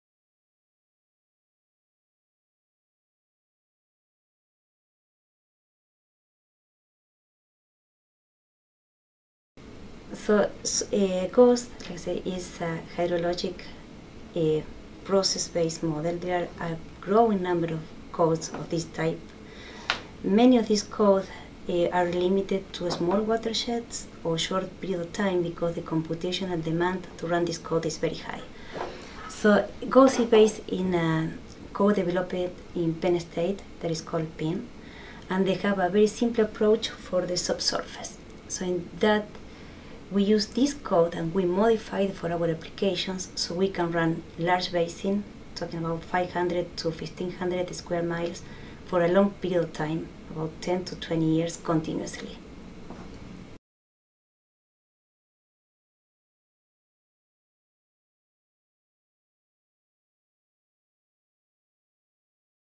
Ask a Scientist - GHOST Modelling
We ask Marcela Politano, of the University of Iowa, the question: How is GHOST different, or unique, relative to other models?